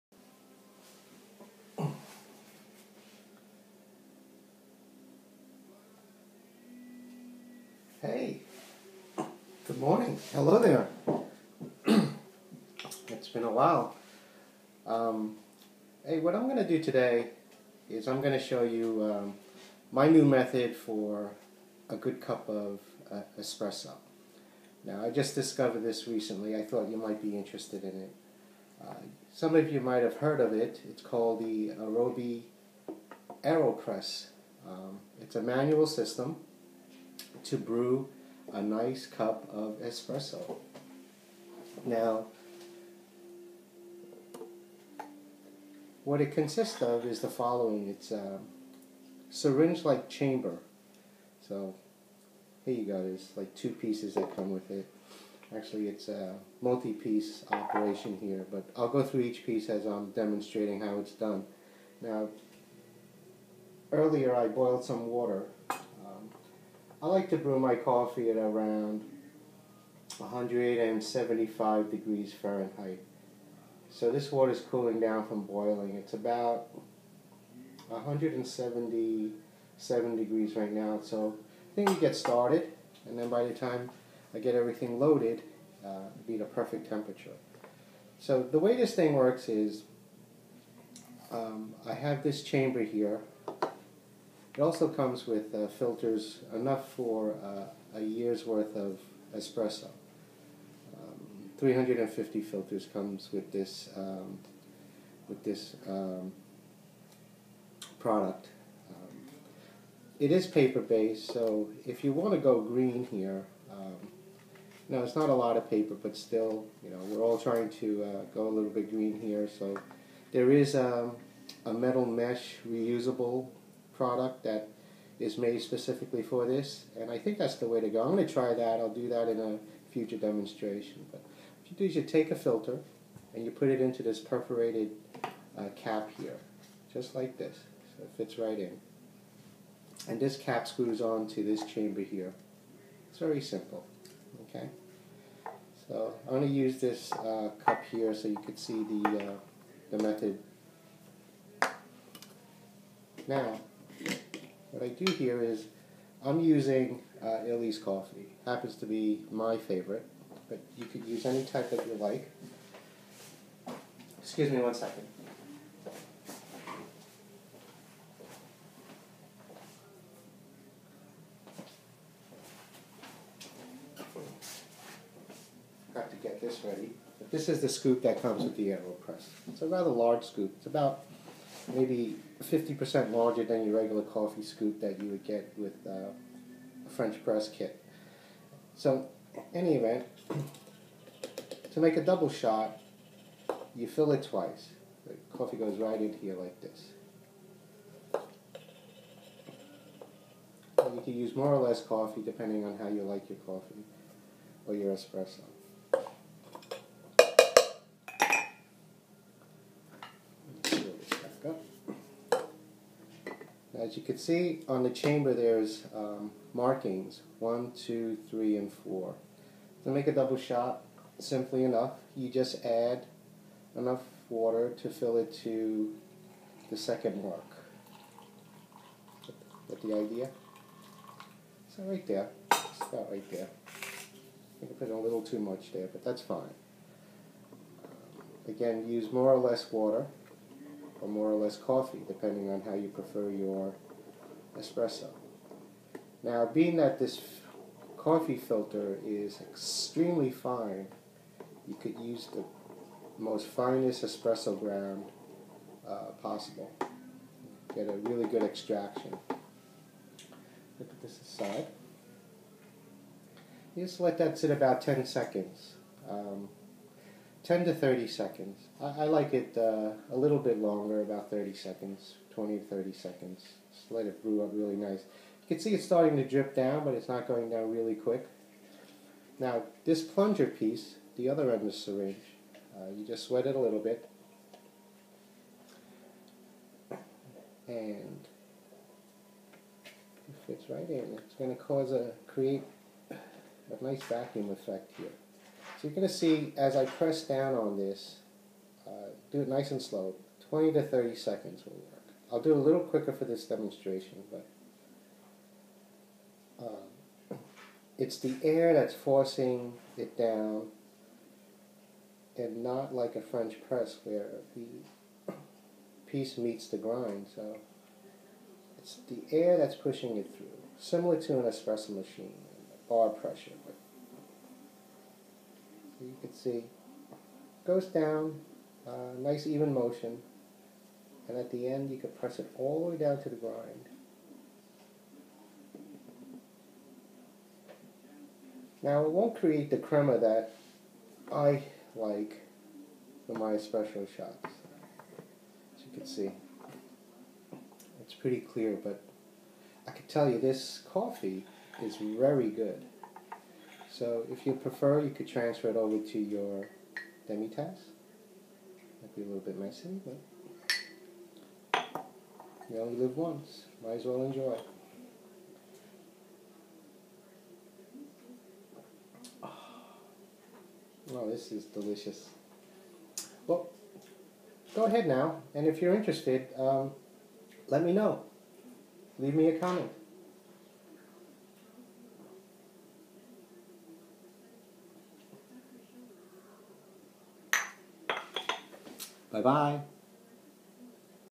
Aeropress
Making a double shot of illy's with an Aeropress